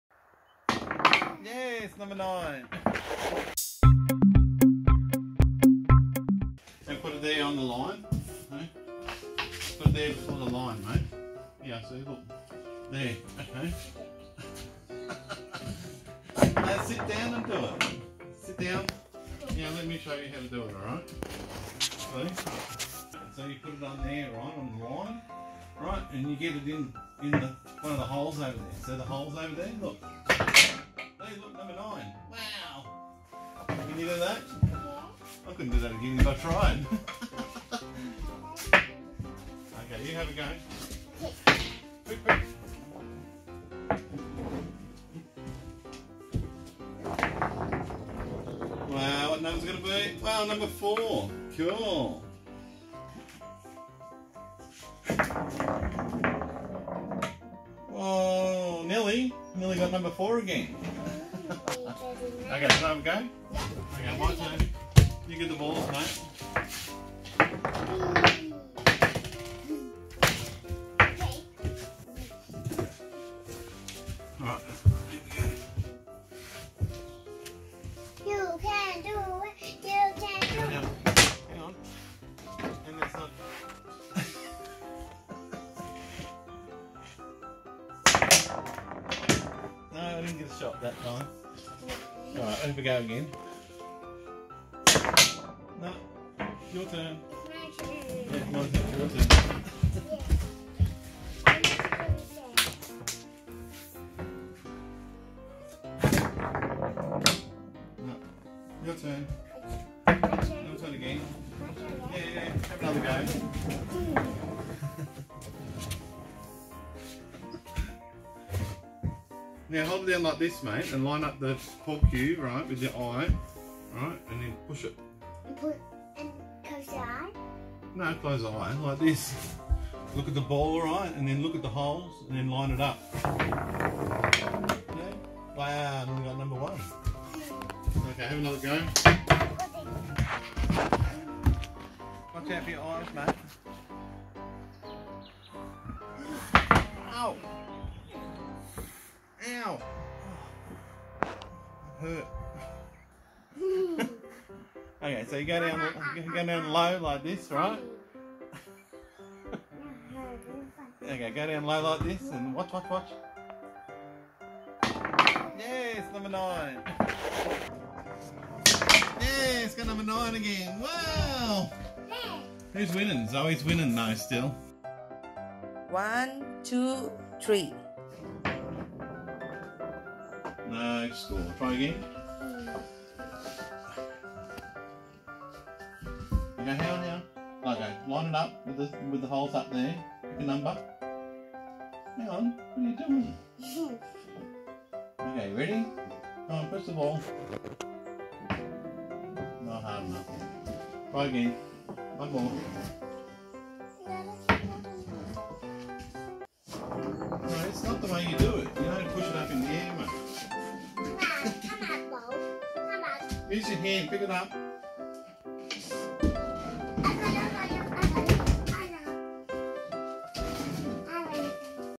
Zoe And Dad Play Bobs An Old Australian Ball Game | Vintage Pool Table
We didn't really know how to play this game but it was fun, bobs is an old Australian game played around the 1930s for young children and adults!